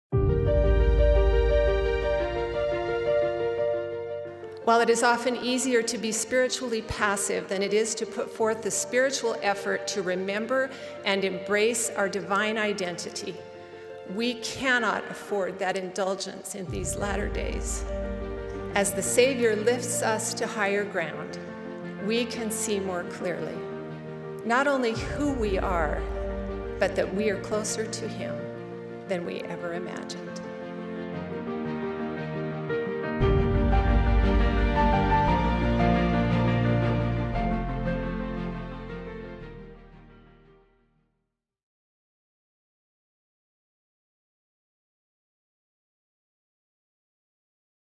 De talade till oss – Joy D. Jones
Lyssna på kyrkans nutida ledare och Jesu Kristi apostlar när de talar under den världsomfattande konferensen i april.